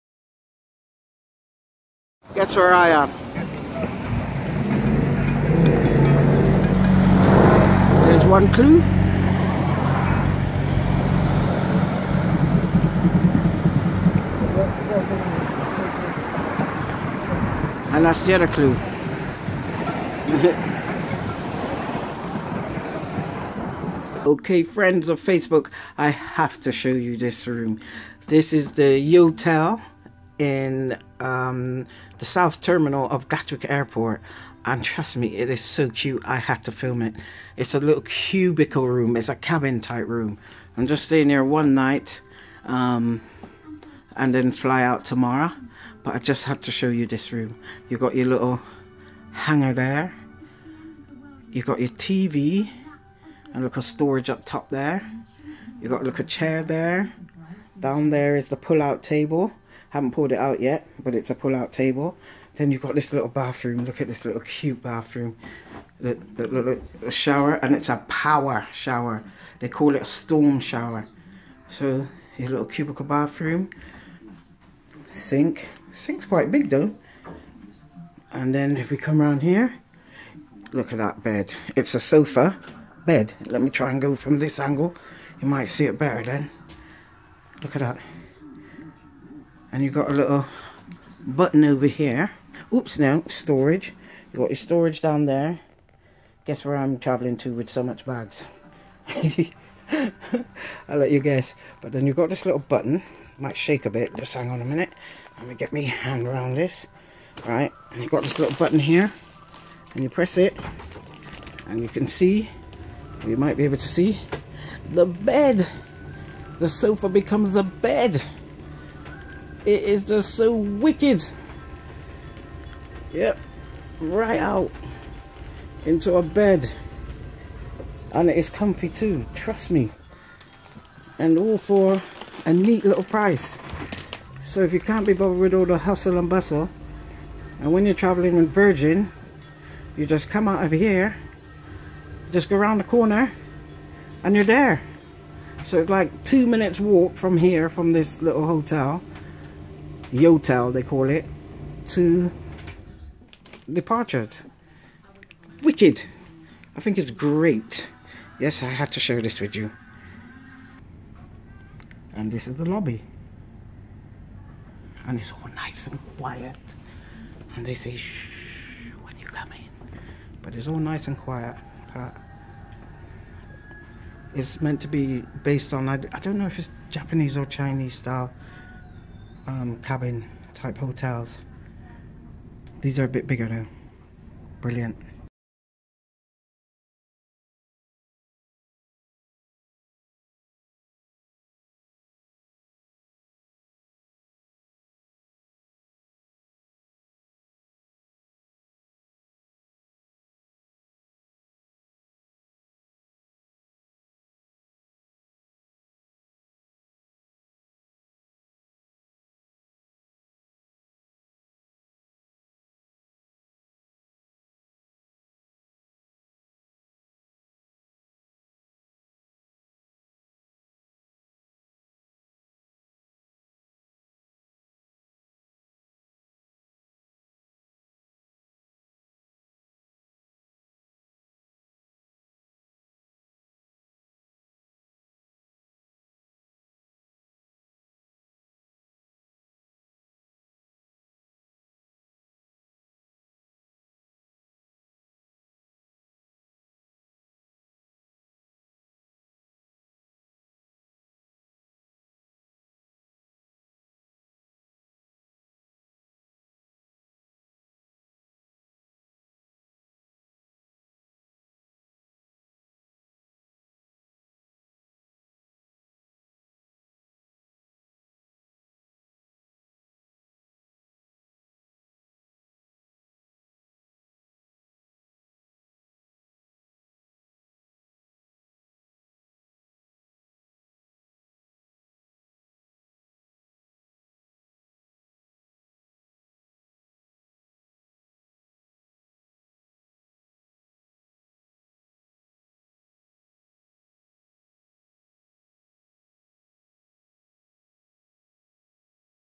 Where Am I UK Nov2016
On my travel I came across this Yotelair cabin/shuttle style hotel in the South Terminal of Gatwick airport and thought I would share my experience with you.